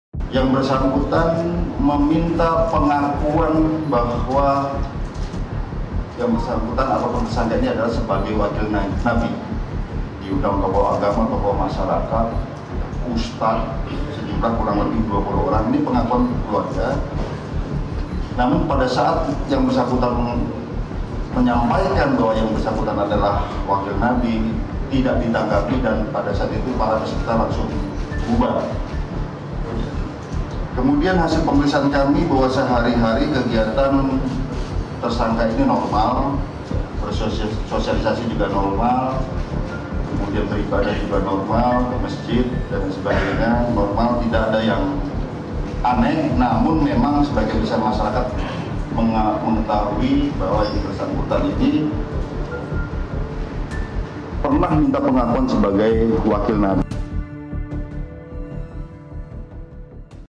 HASIL PEMERIKSAAN POLISI MUSTOFA DINILAI ORANG YAnG NORMAL DAN SERING BEROSIALISASI
Direskrimum Polda Metro Jaya, Kombes Pol Hengki Haryadi menyatakan Mustofa, pelaku p3nemb4kan di MUI pernah mengaku sebagai wakil nabi. Dan dari hasil pemeriksaan bahwa keseharian tersangka layaknya sebagai orang normal dan sering bersosialisasi.